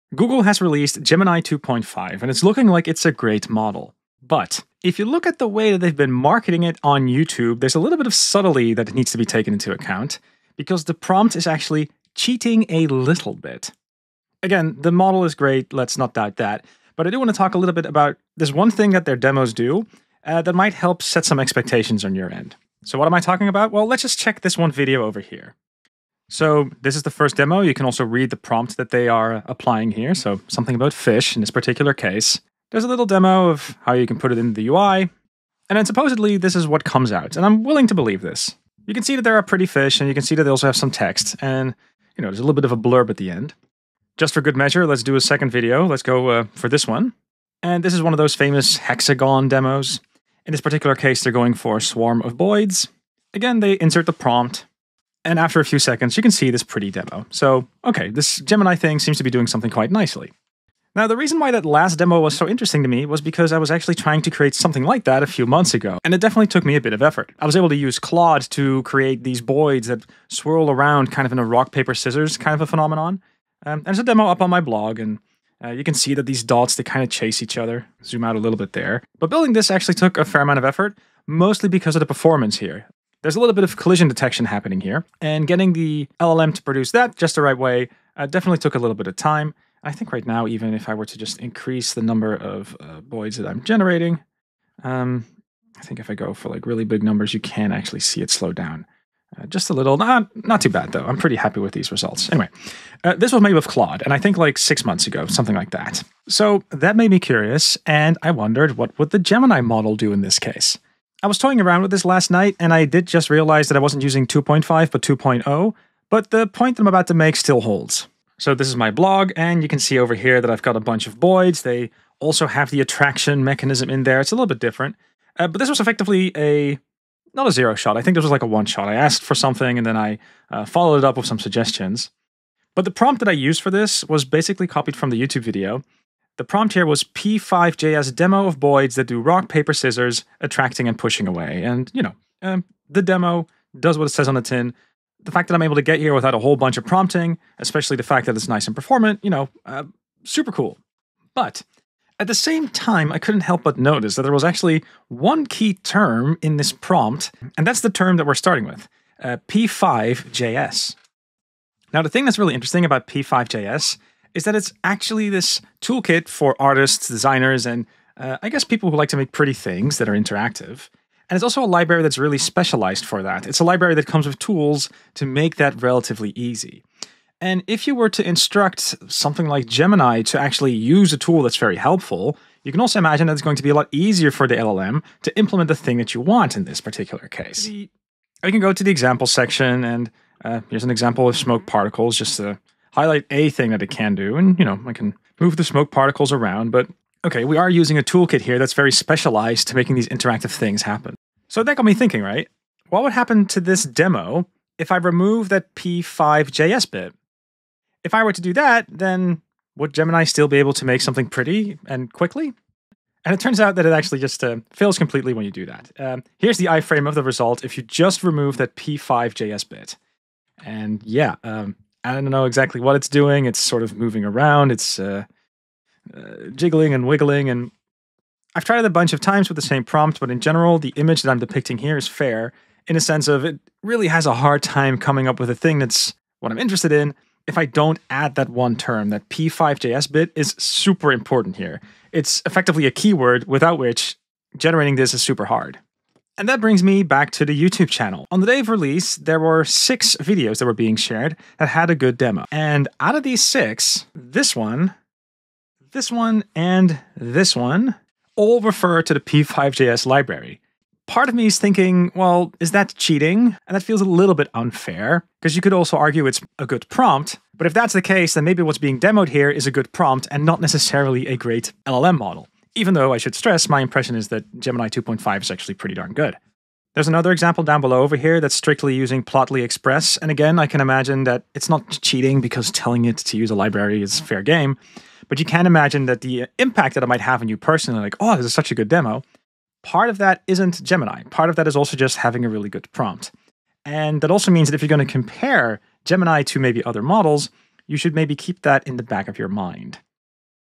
Gemini 2.5: good demo, or a good prompt?
If you pay attention to the deepmind YT account then you may notice an interesting detail. Three of the six demos all use this one token and, as we will explain in this video, it might be the reason why they all demo so well.